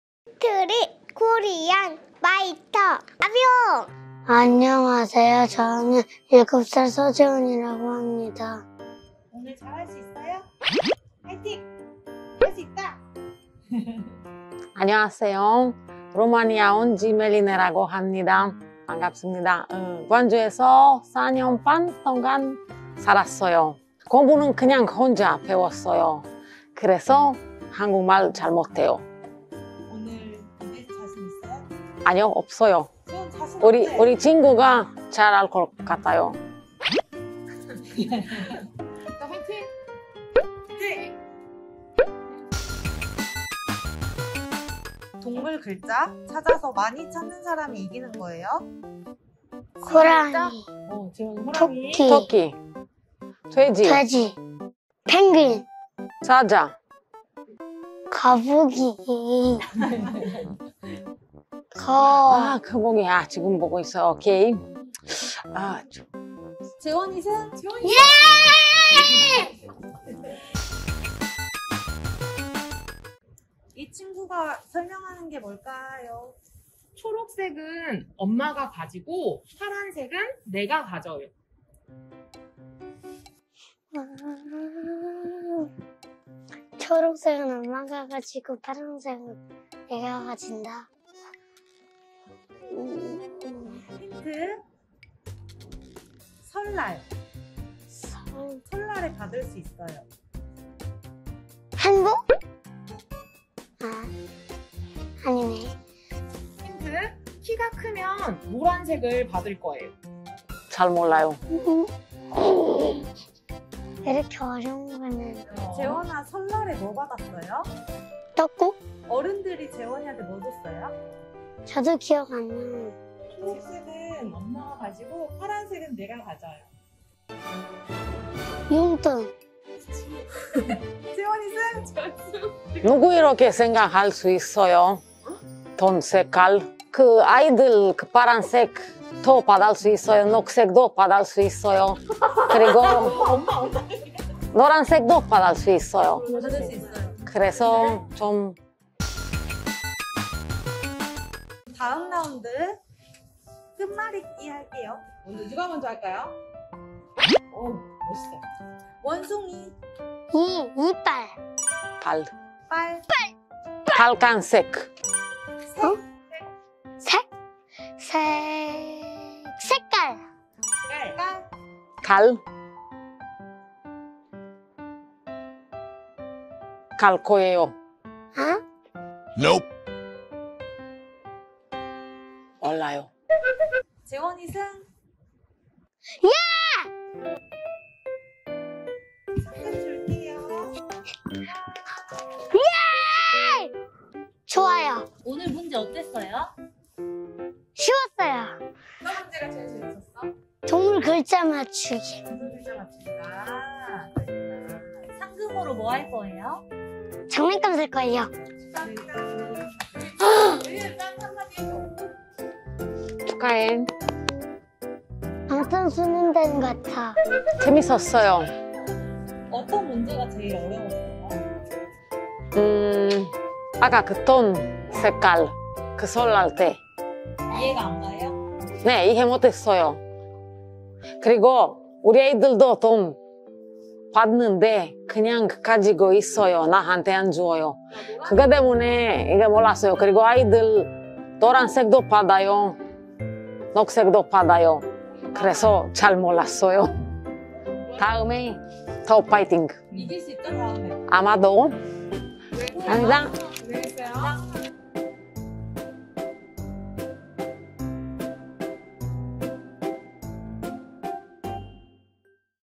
스코파 ep.11 (Street Korean Fighter)
한국에서 살고 있는 외국인과
한국의 어린아이 중 누가 더 한국어를 잘 할까요?

루마니아에서 온 BTS 같은 광주살이 4년차 멜리네와
7살 재원이의 한국어 대결 과연 누가 이길까요?

함께 보시죠!!!

장소제공 : 국립아시아문화전당 어린이문화원